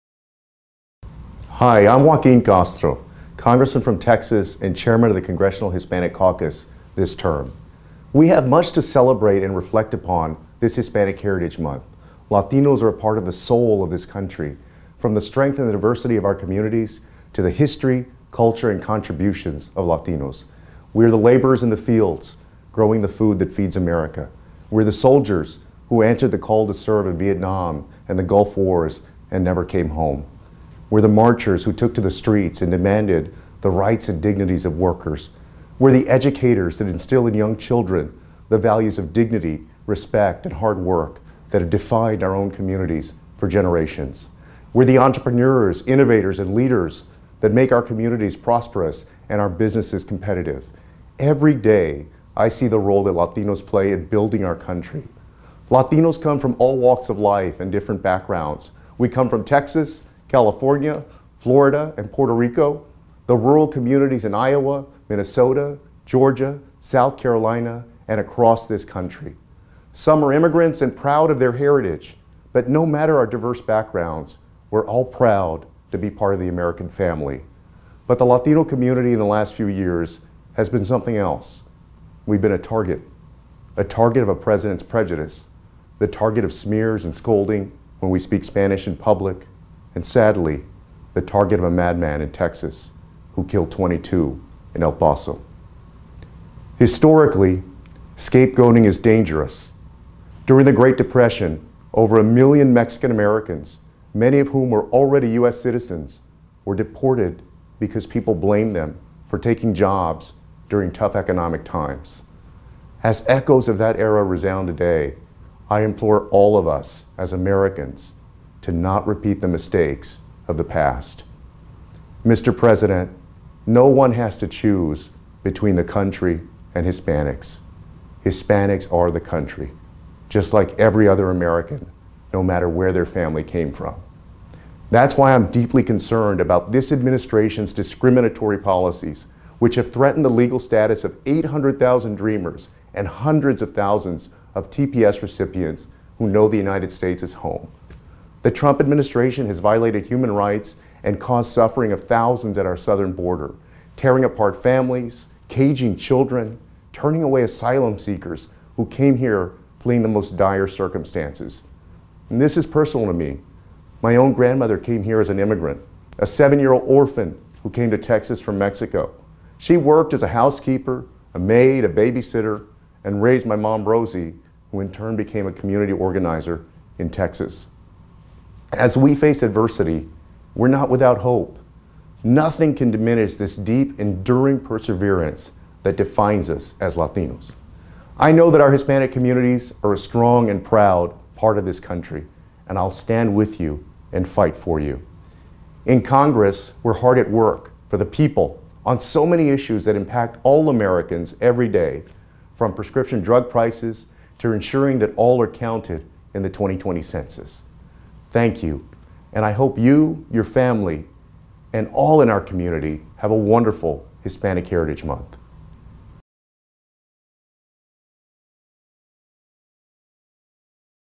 Weekly Democratic Debate -- Congressman Joaquin Castro
In this week’s address, Congressman Joaquin Castro discussed the celebration of Hispanic Heritage month and the work House Democrats are doing For The People to counter the discriminatory and harmful policies of the Trump Administration.

Below is a full transcript of the address:

“Hello, I’m Joaquin Castro, Congressman from Texas and Chairman of the Congressional Hispanic Caucus this term.

“We have much to celebrate and reflect upon this Hispanic Heritage Month.  Latinos are a part of the soul of this country – from the strength and diversity of our communities, to the history, culture and contributions of Latinos.

“We are the laborers in the fields, growing the food that feeds America.

“We are the soldiers who answered the call to serve in Vietnam and the Gulf Wars and never came home.

“We are the marchers who took to the streets and demanded the rights and dignities of workers.

“We are the educators that instill in young children the values of dignity, respect, and hard work that have defined our own communities for generations.

“We are the entrepreneurs, innovators, and leaders that make our communities prosperous and our businesses competitive.

“Every day, I see the role that Latinos play in building our country.

“Latinos come from all walks of life and different backgrounds.  We come from Texas, California, Florida and Puerto Rico, the rural communities in Iowa, Minnesota, Georgia, South Carolina, and across this country.

“Some are immigrants and proud of their heritage, but no matter our diverse backgrounds, we are all proud to be a part of the American family.

“But the Latino community, in the last few years, has been something else.

“We have been a target – a target of a president’s prejudice, the target of smears and scolding when we speak Spanish in public.  And sadly, the target of a mad man in Texas, who killed 22 in El Paso.

“Historically, scapegoating is dangerous.  During the Great Depression, over a million Mexican Americans, many of whom were already US citizens, were deported because people blamed them for taking jobs during tough economic times.

“As echoes of that era resound today, I implore all of us as Americans to not repeat the mistakes of the past.

“Mr. President: No one has to choose between the country and Hispanics.

“Hispanics are the country, just like every other American, no matter where their family came from.

“That is why I am deeply concerned about this Administration’s discriminatory policies – which have threatened the legal status of 800,000 Dreamers and hundreds of thousands of TPS recipients who know the United States as home.

“The Trump Administration has violated human rights and caused suffering of thousands at our Southern Border – tearing apart families, caging children, turning away asylum seekers who came here fleeing the most unimaginable circumstances.

“This is personal to me – my own grandmother came here as an immigrant, a seven year old orphan who came to Texas from Mexico.  She worked as a housekeeper, maid, a babysitter, and raised my mother, Rosie, who in turn became a community organizer in Texas.

“As we face adversity, we are not without hope.  Nothing can diminish the deep, enduring perseverance that defines us as Latinos.

“I know that our Hispanic communities are a strong and proud part of this country, and I will stand with you and fight for you.

“In Congress, we are hard at work For The People on so many issues that impact all Americans every day, from prescription drug prices to ensuring that all are counted in the 2020 Census.

“Thank you, and I hope you, your family, and all in our community have a wonderful Hispanic Heritage Month.”